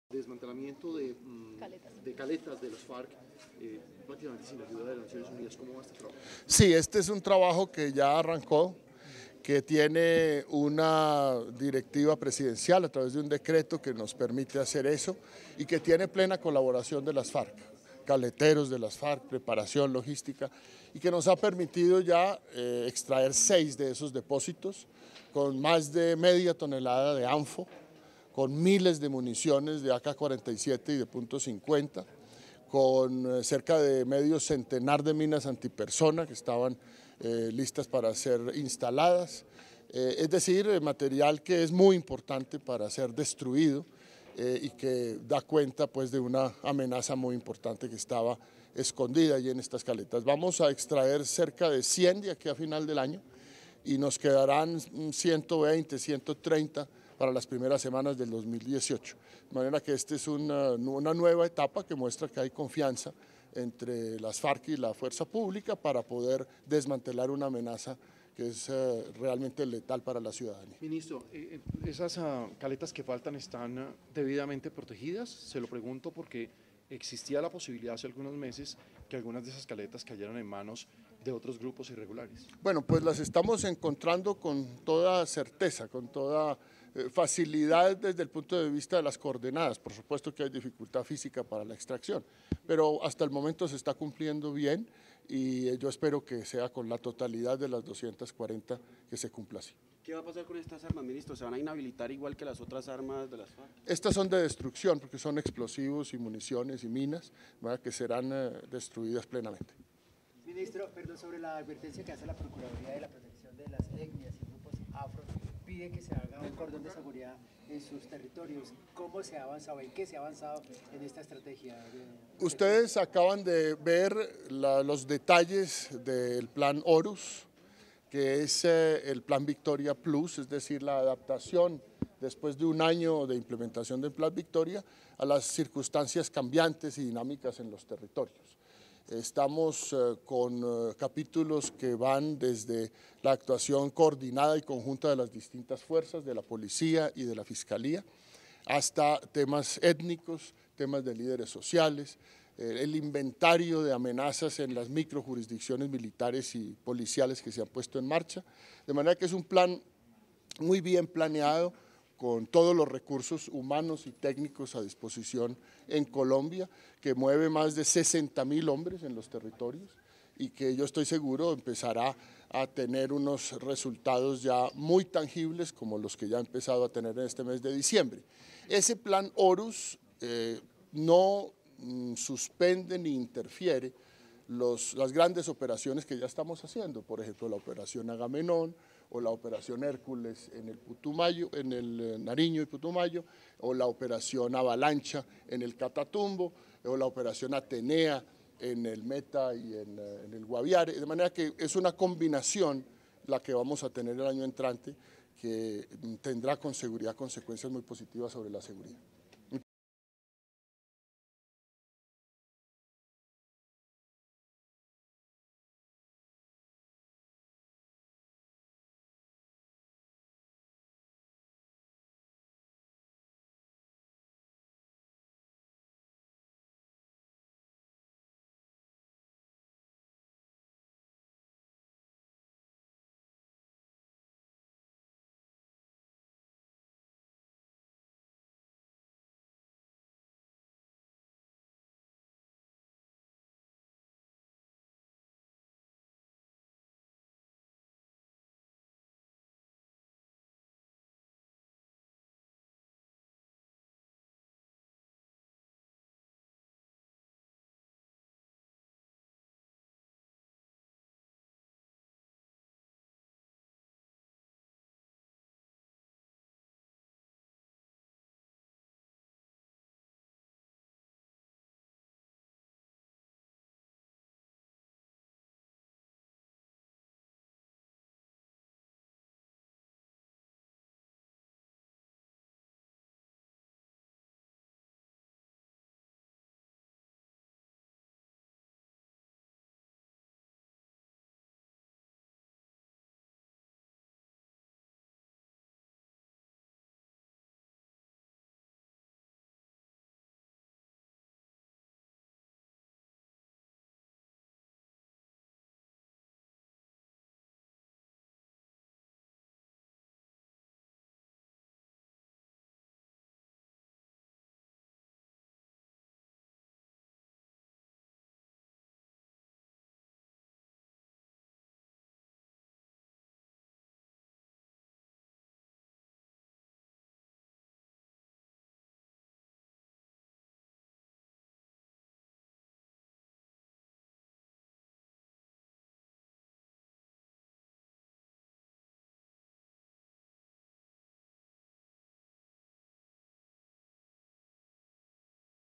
Fuerzas Militares continúan operaciones para extracción de caletas de las Farc
Más de media tonelada de explosivo anfo, munición de diferente calibre y cerca de 50 minas antipersona hacen parte del material incautado por las Fuerzas Militares en el marco de operaciones especiales para la ubicación y neutralización de caletas en el país.

“Este es un trabajo que ya arrancó y tiene una directiva presidencial a través de un decreto que nos permite hacer eso, que tiene plena colaboración de las Farc y que nos ha permitido extraer seis de esos depósitos, con más de media tonelada de anfo, miles de municiones de AK 47 y Punto 50 y  cerca de medio centenar de minas antipersona”, informó éste miércoles el ministro de Defensa, Luis Carlos Villegas.

El ministro reveló además, que antes de finalizar el año se extraerán cerca de 100 caletas más y en los primeros meses de 2018, entre 120 y 130: “Las estamos encontrando con toda certeza y facilidad desde el punto de vista de las coordenadas y por supuesto hay dificultad física para la extracción, pero hasta el momento se está cumpliendo bien y yo espero que se cumpla de esta manera con la totalidad de las 240”.

Villegas señaló que el material encontrado en estas caletas se destruirá por tratarse de explosivos y destacó que el trabajo con las Farc es gracias a la confianza adquirida durante el proceso. “Esta es una nueva etapa que muestra que hay confianza entre la Fuerza Pública y las Farc para poder desmantelar una amenaza que es realmente letal para la ciudadanía”, concluyó.